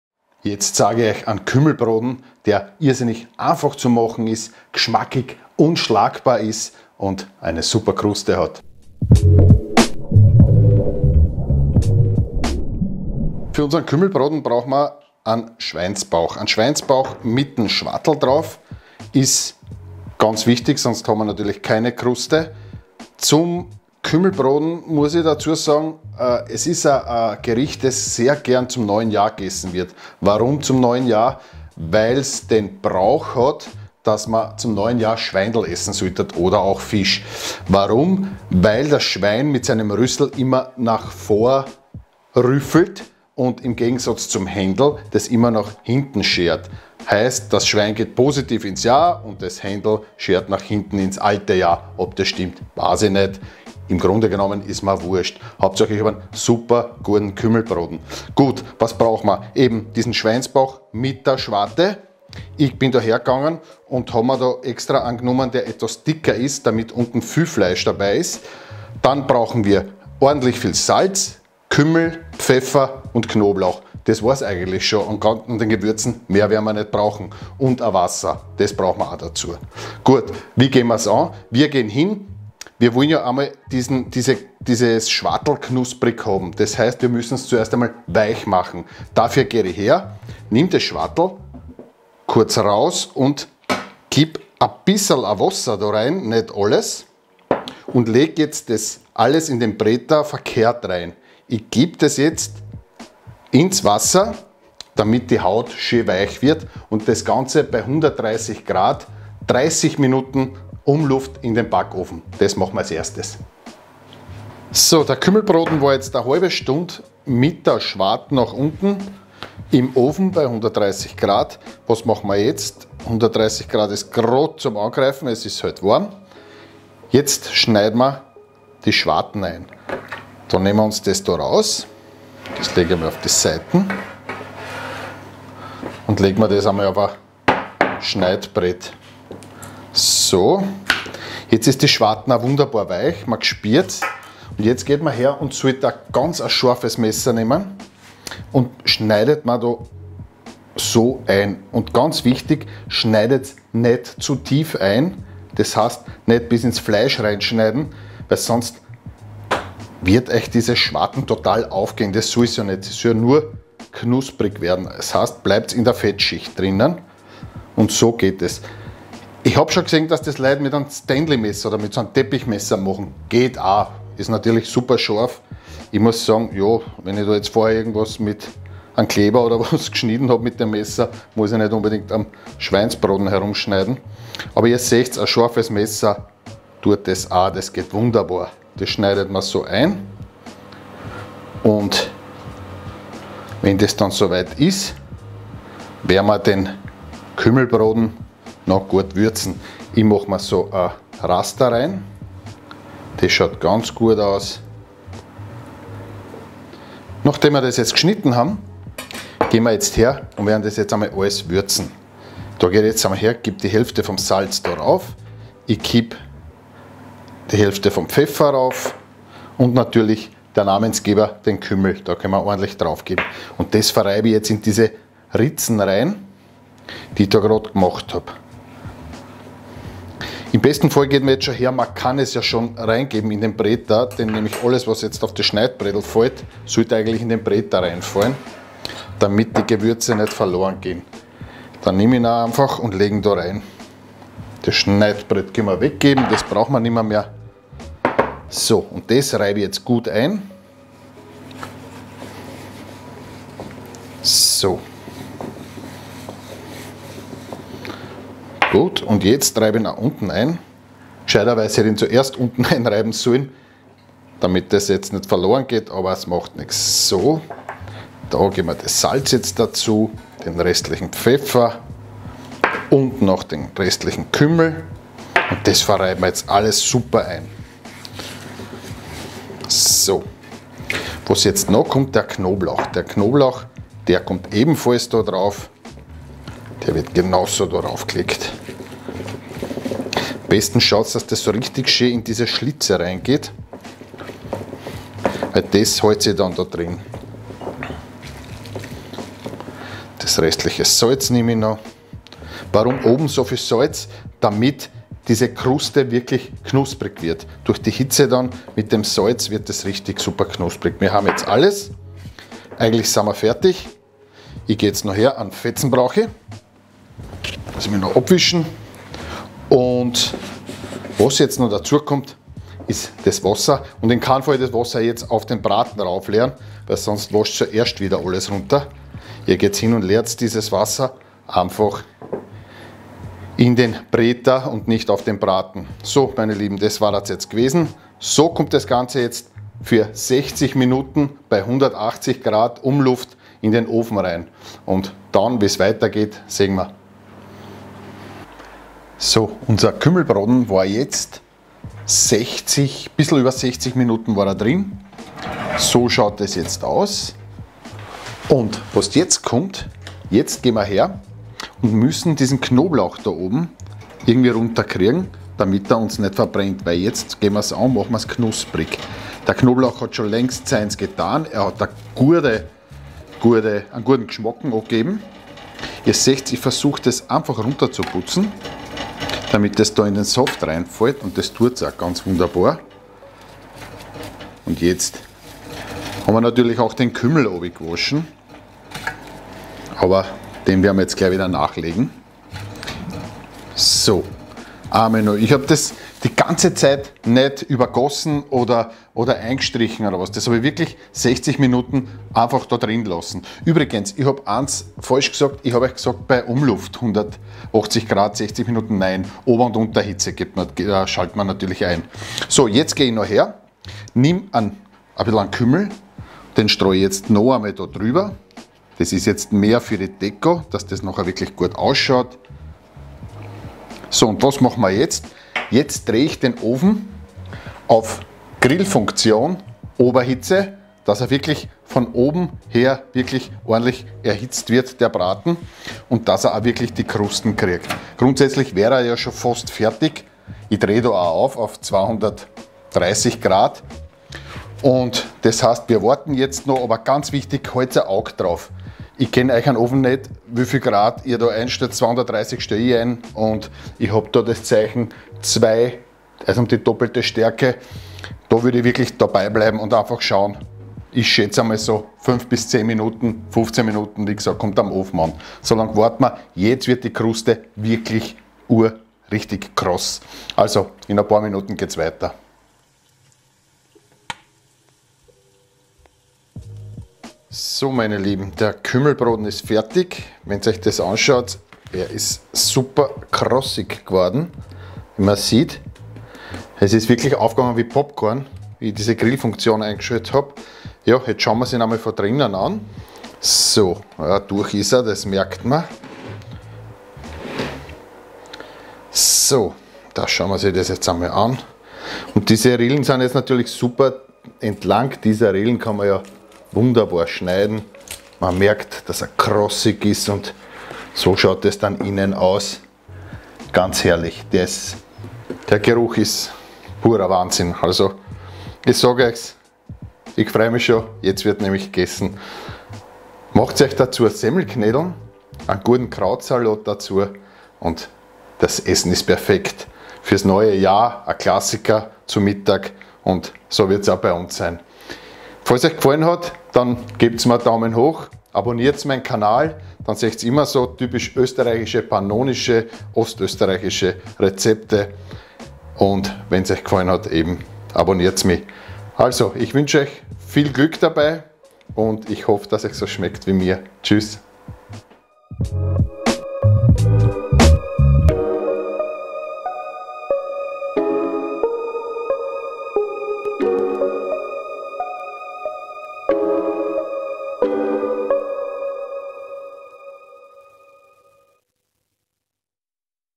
Echt krosser Kümmelbraten / Schweinsbraten
Der Kümmelbraten ist ein Schweinsbraten vom Schweinebauch mit einer knusprigen Kruste.
Rezept:
1,5-2kg Bauchfleisch
2-3TL Salz
3TL Kümmel ganz
2TL Pfeffer
3-4 Knoblauchzehen
500g Wasser